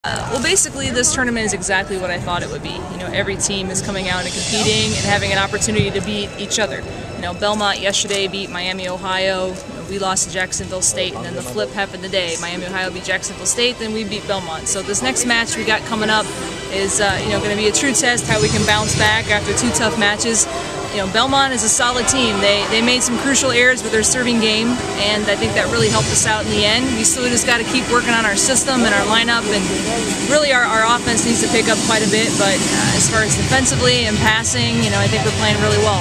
April Jauregui Post Game Interview vs. Belmont
University of Memphis volleyball head coach April Jauregui's post game interview following a 3-2 victory over Belmont in the Belmont Invitational in Nashville, Tenn., on August 28, 2010